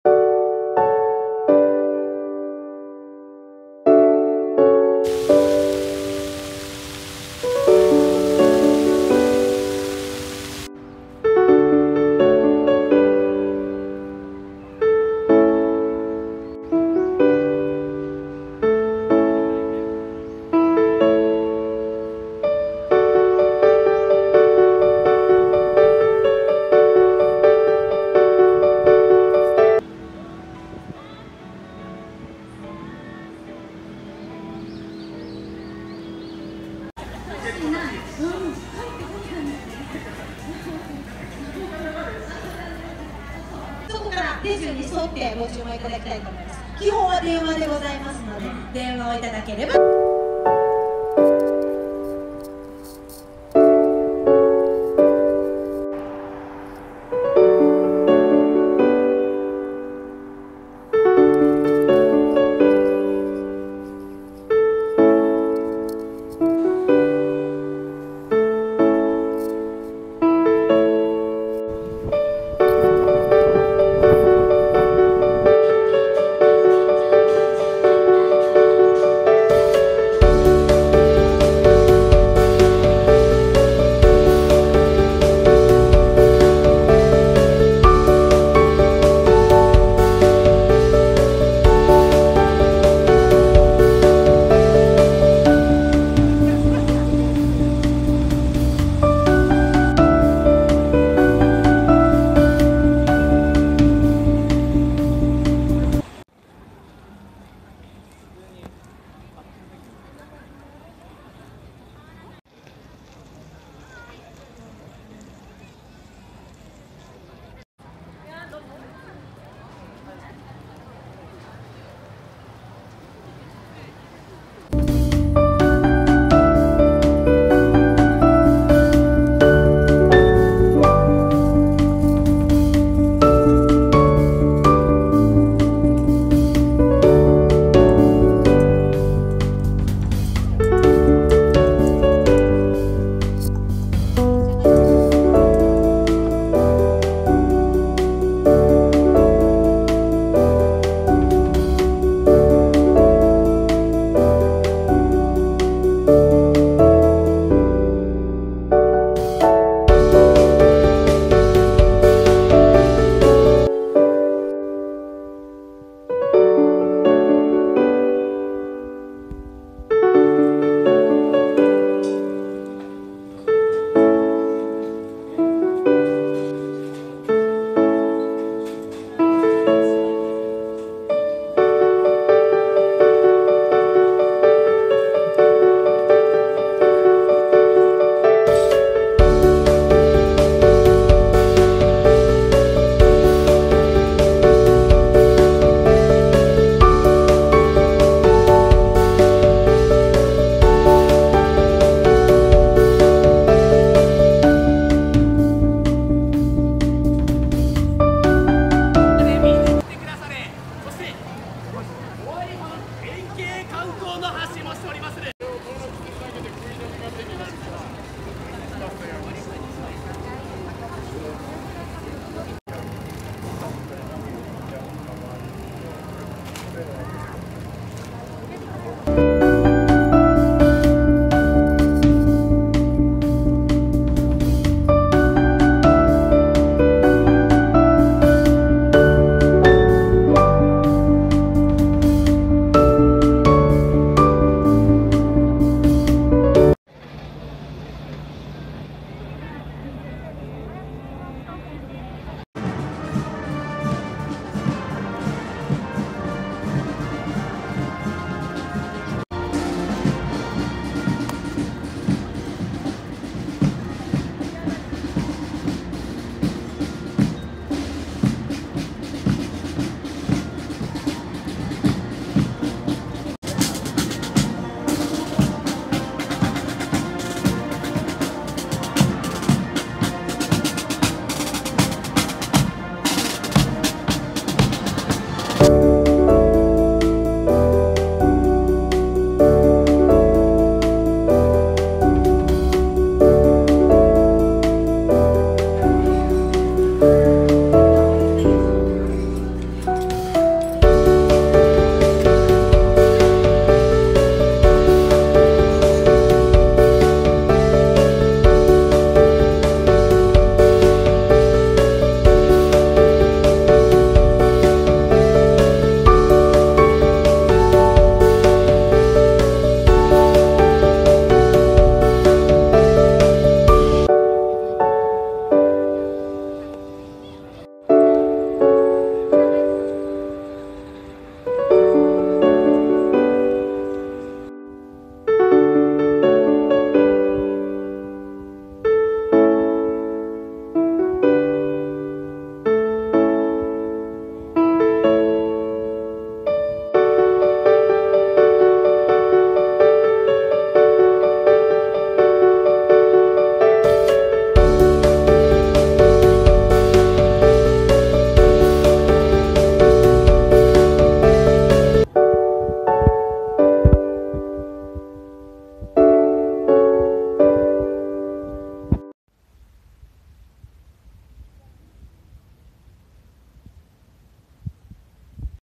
Hisaya-odori Park in Nagoya City Japan...A nice place to eat, relax, shop and play!!!
Hisaya-odori Park in Nagoya City, Japan has a lot to offer . Enjoy eating and drinking with your family and friends. You can also enjoy shopping while you’re in the park . You can take photos of the Nagoya TV  Tower and the Oasis 21 standing across this park. When you’re in Nagoya these are some of the bests  spots to visit.